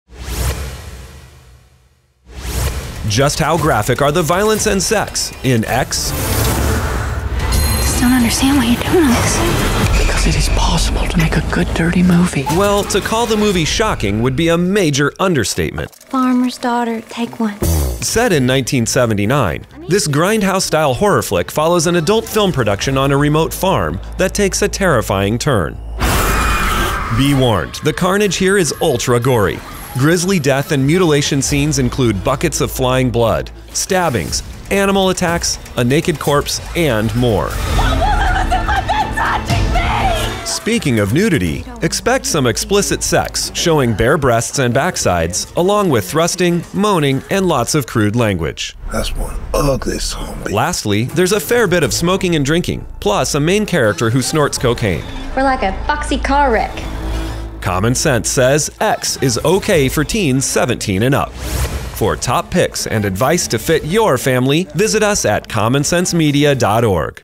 Just how graphic are the violence and sex in X? | Common Sense Movie Minute
Our Stress-Free App for Finding The Best Kids’ Entertainment Is X okay for your family? Watch our review to find out what Common Sense says about violence, scariness, sexy stuff, language, drinking and drugs.

Cast: Mia Goth, Jenna Ortega, Brittany Snow
Director: Ti West
Studio: A24
MPAA rating: R